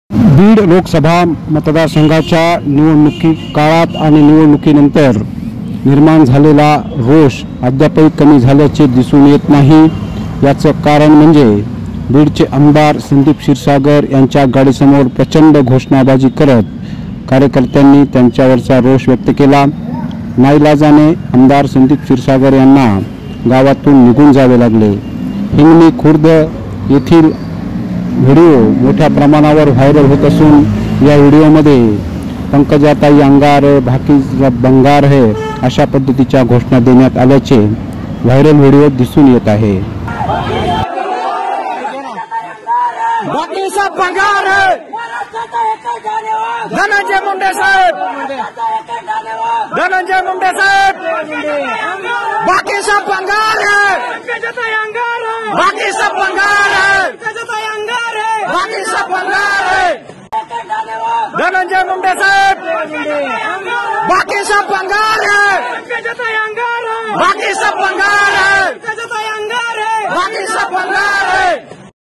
Viral Video: आ.संदीप क्षीरसागरांच्या गाडीसमोर जोरदार घोषणाबाजी | गावकऱ्यांचा रोष | निघून जावे लागले
Viral Video: आ.संदीप क्षीरसागरांच्या गाडीसमोर जोरदार घोषणाबाजी | गावकऱ्यांचा रोष | गावातून निघून जावे लागले